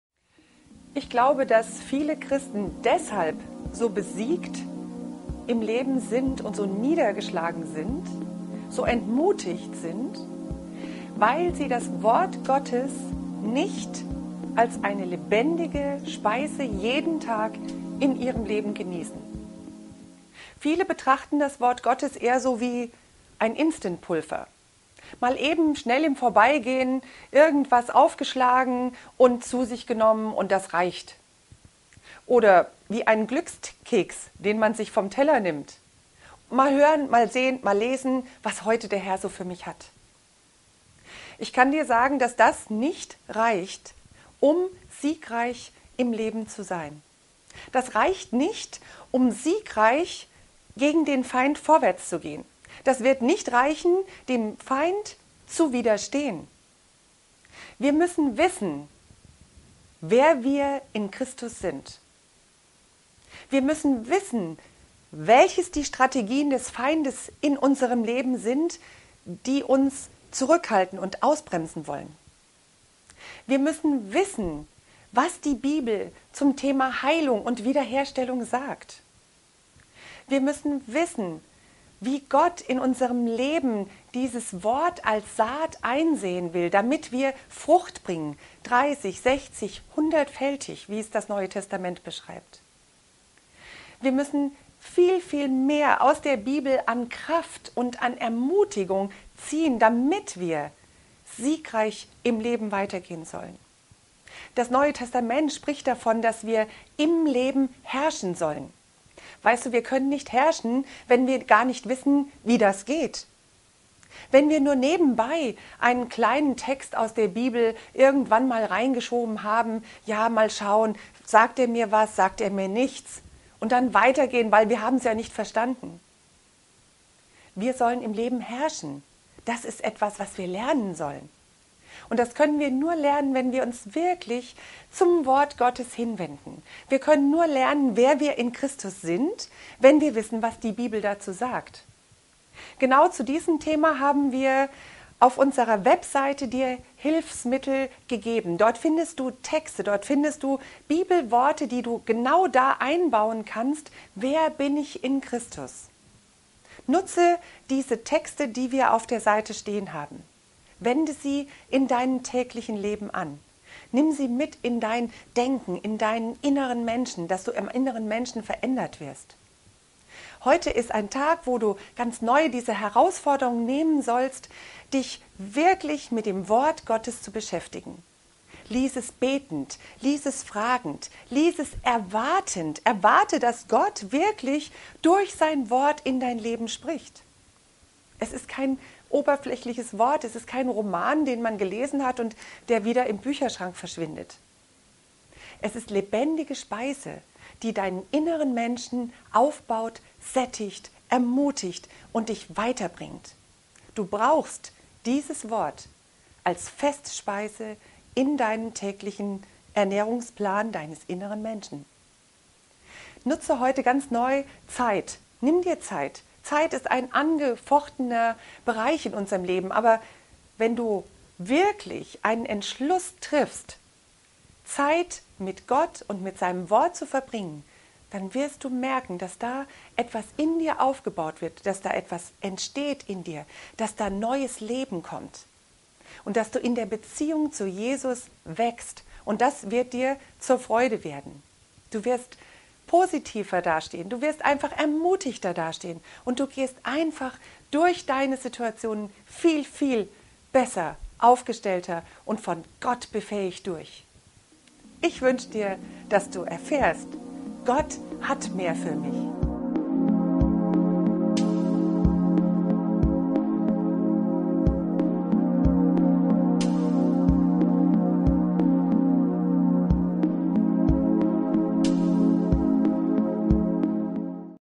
Gottes Wort als Fast Food?
Ich glaube, dass viele Christen deshalb so besiegt durch ihr Leben gehen, weil sie zu wenig das lebendige, lebensverändernde Wort Gottes in sich tragen. Das Wort Gottes ist kein Instantpulver, das man im Vorbeigehen schnell zu sich nimmt, kein Glückskeks, kein Fast Food. Wenn du auf einem Marsch durch die Hitze bist, wirst du sicher auch nicht mit einem kleinen Schluck aus der Feldflasche zufrieden sein. Du brauchst mehr, um ans Ziel zu kommen.

Wir brauchen die „feste Speise", die das Wort Gottes für uns hat, um in diesem Leben wirklich vorwärts zu kommen. Wir müssen wissen, wer wir in Christus sind, welche Identität und Position wir in ihm haben. Wir müssen wissen, was die Bibel über Heilung und Wiederherstellung sagt und wie wir verheißungsorientiert leben.

Das alles finden wir in der Bibel. Doch du wirst es nicht im Vorbeigehen entdecken. Du musst eine Entscheidung dafür treffen, dir Zeit für Gottes Wort zu nehmen. Beginne, Worte aus der Bibel, die dir persönlich wichtig sind, aufzuschreiben. Trage sie mit dir, studiere sie, sprich sie aus über deinem Leben, über bedrängende Gedanken und Situationen.

In einem anderen Clip haben wir darüber gesprochen, dass wir die „Verheißungen-Gottes-Sprache" genauso lernen müssen, wie man eine Sprache lernt. Das bedeutet eben auch üben und lernen. Aber es wird sich überaus lohnend auf dein Leben auswirken.

Fanden sich Worte von dir, dann habe ich sie gegessen, und deine Worte waren mir zur Wonne und zur Freude meines Herzens; denn dein Name ist über mir ausgerufen, HERR, Gott der Heerscharen.
Jeremia 15,16